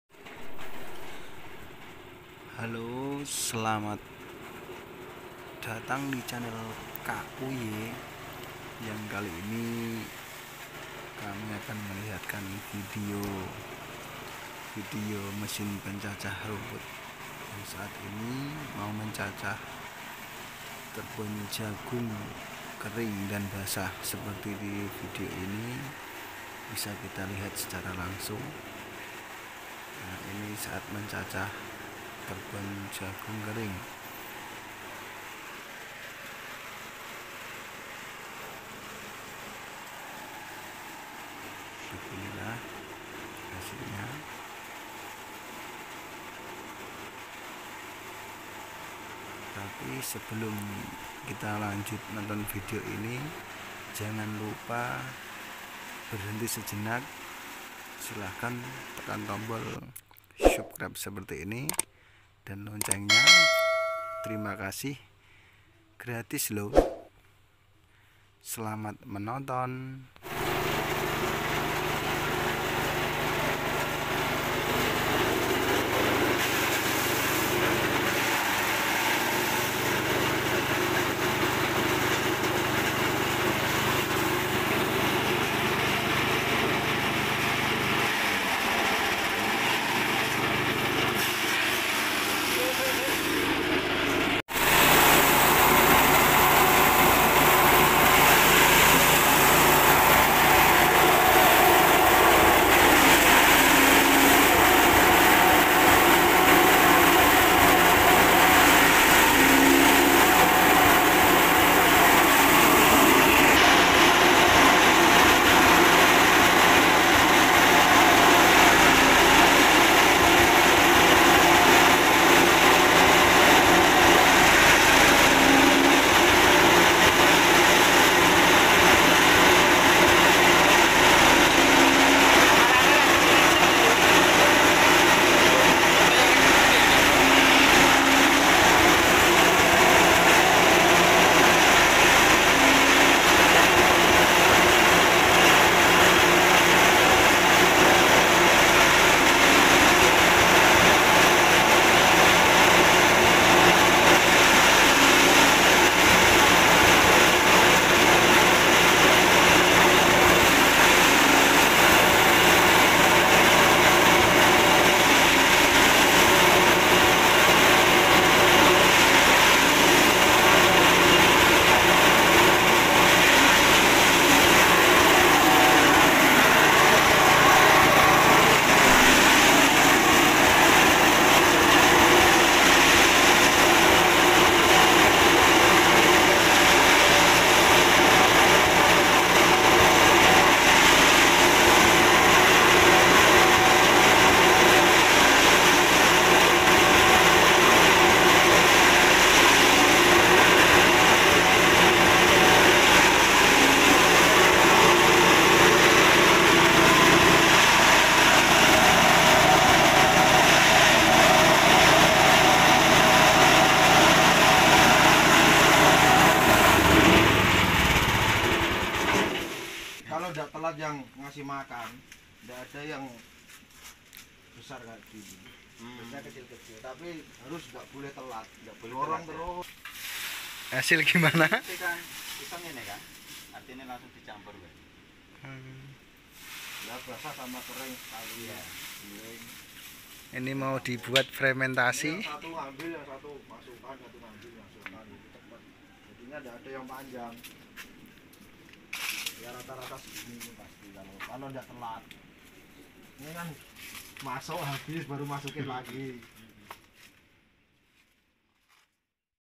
COPER MESIN PENCACAH TEBON BASAH / KERING DAN RUMPUT LAIN NYA BUAT PAKAN SAPI / KAMBING
Alat ini udah di lengkapi sistem roler & pisau 6 biji yang sangat tajam & alat ini di putar dengan dukungan mesin diesel berbahan bakar bensin 6,5.PK jadi kuat untuk mencacah. Terimakasih di sini melayani pengiriman sampe luar jawa. Info harga lebih lanjut. Hp/wa. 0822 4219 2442. smoga ini bermanfaat bagi peternak indonesia bisa memper mudah untuk mencacah pakan ternak sapi & kambing.  Terimakasih smoga peternak indonesia semakin jaya selalu.